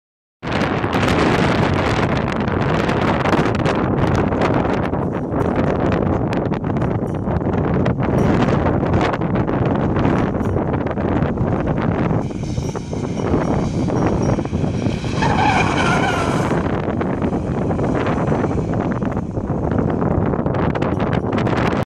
Now That's How to J-Turn!
How to J Turn. ONLY try this on a track, and preferably not in a new car. Best done on a track day. They will have their own cars for you to use. Book a day or a couple of hours at your local track.

1. Reverse back at speed.
2. Clutch down off gas. Don't touch the brake! On Auto just come off the gas.
3. Steer sharply 360 degrees (1 turn) Left.
4. Steer sharply 360 degrees (1 turn) Right, back to the straight position JUST BEFORE the car lines up in a straight line.
5. Change into 2nd gear whilst turning. On Auto go back into drive.
6. Gas and clutch up to keep the car moving. On Auto just go back on the gas. Courtesy of